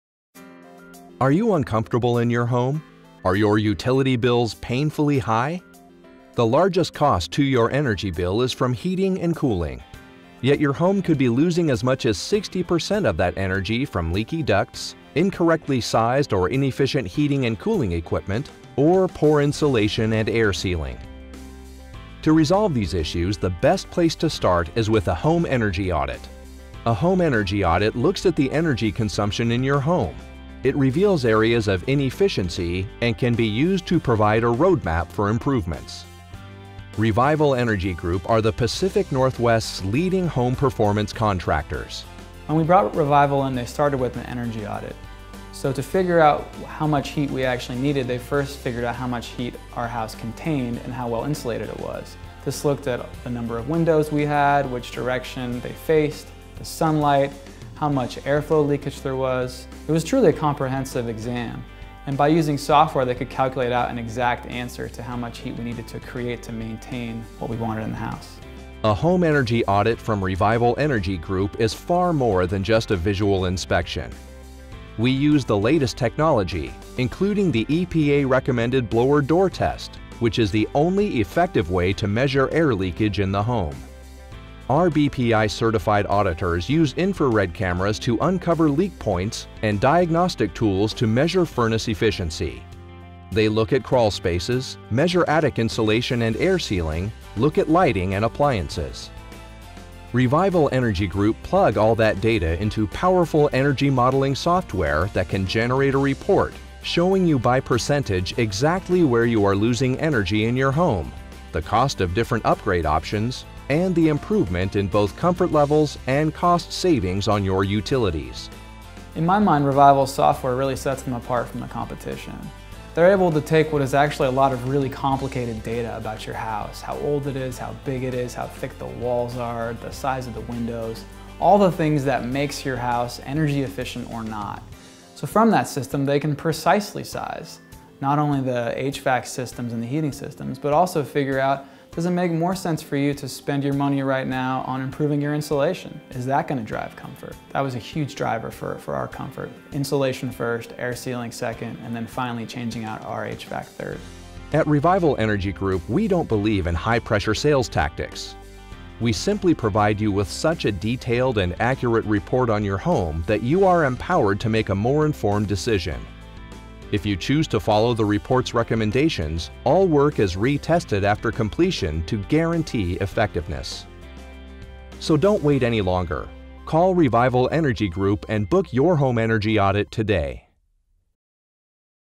Home Energy Audit
Are you uncomfortable in your home?  Are your utility bills painfully high? The largest cost to your energy bill is from heating and cooling.  Yet your home could be losing as much as sixty percent of that energy from leaky ducts, incorrectly sized or inefficient heating and cooling equipment or poor insulation and air sealing.

To resolve these issues the best place to start is with a home energy audit.

A home energy audit looks at the energy consumption in your home.  It reveals areas of inefficiency and can be used to provide a roadmap for improvements.

Revival Energy Group are the Pacific North West’s leading home performance contractors.

A home energy audit from Revival Energy Group is far more than just a visual inspection. We use the latest technology, including the EPA recommended blower door test, which is the only effective way to measure air leakage in the home.

Our BPI certified auditors use infared cameras to uncover leak points and diagnostic tools to measure furnace efficiency.  They look at crawl spaces, measure attic insulation and air sealing, look at lighting and appliances.

Revival Energy Group plug all that data into powerful energy modeling software that can generate a report showing you by percentage exactly where you are losing energy in your home; the cost of different upgrade options, and the improvement in both comfort levels and cost savings on your utilities.

If you choose to follow the report’s recommendations all work is re-rested after completion to guarantee effectiveness.

So don’t wait any longer, call Revival Energy Group and book your home energy audit today.